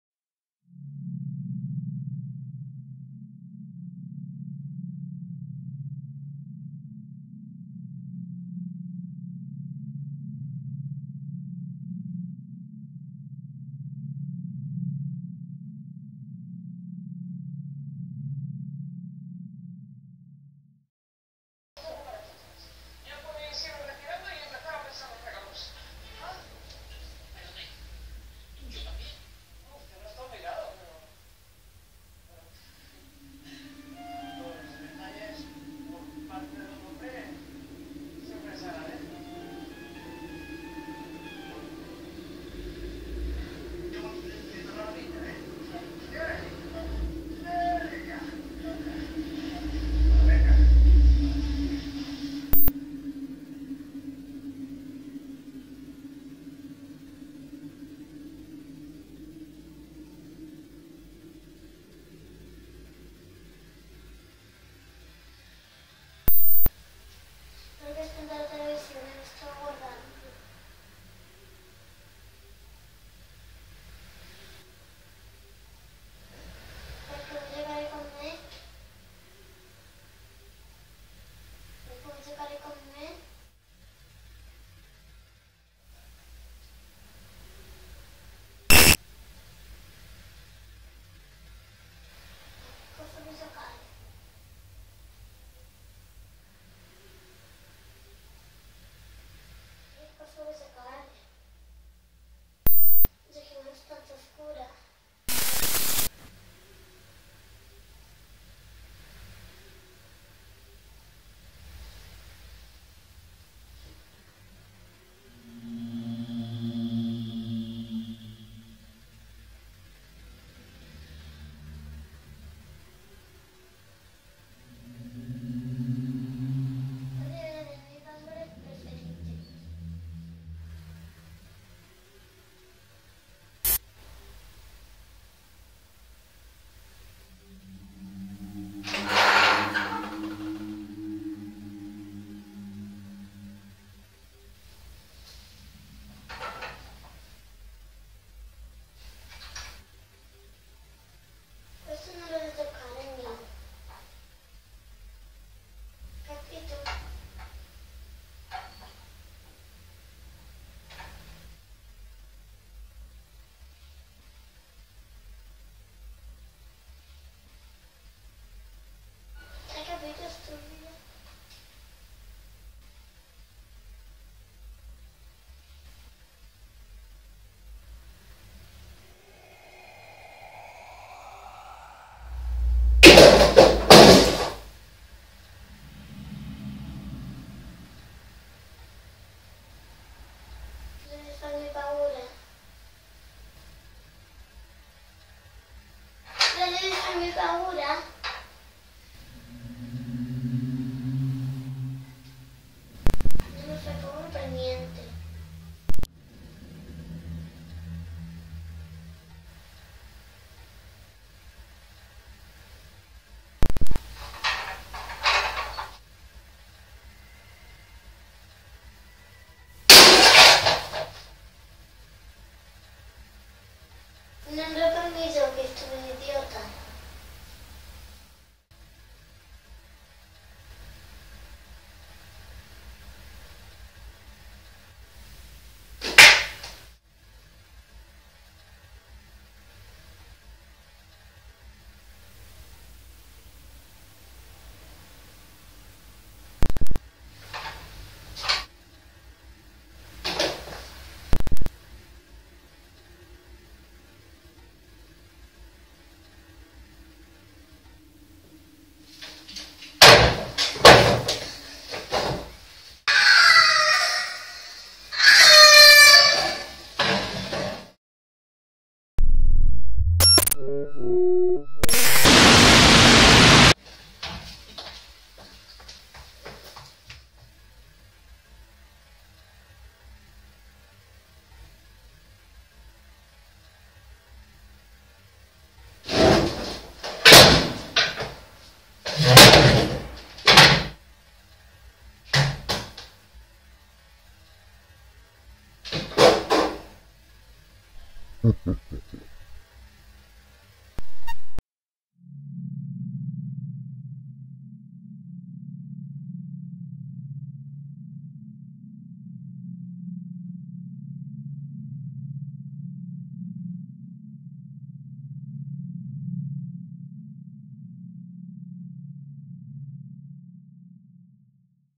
Niña pelea con un fantasma que la atormenta, y esto se pone muy enojado ...
"El amigo Imaginario"  - serie 2
(Siete Hipótesis)

Observación 2_1
Paranormal Activity:
Terror extremo increible!
Nunca provoques una entidad paranormal!
Nunca juegues con ella porque no sabes a quién te enfrentas.
Podría ser un espíritu simple, o podría ser una entidad maligna, y las consecuencias podrían ser muy graves ...
Este video es una prueba de esto.

Mai provocare un entità paranormale, le conseguenze potrebbero essere molto gravi...